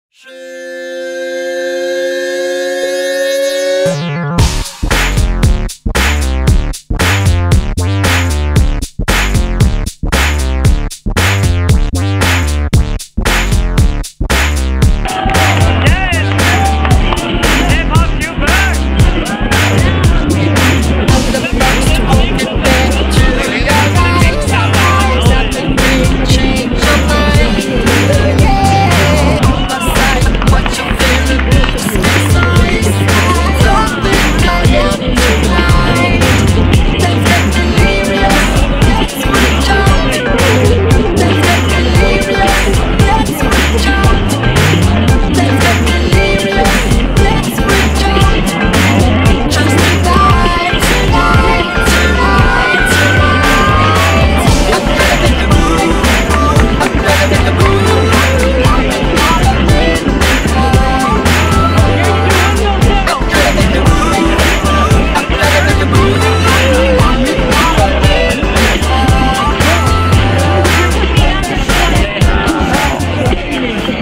AX2015 dance circle day 3 featuring hip hop storm trooper 7-4-2015
AX2015 featuring hip hop storm trooper in the dance floor.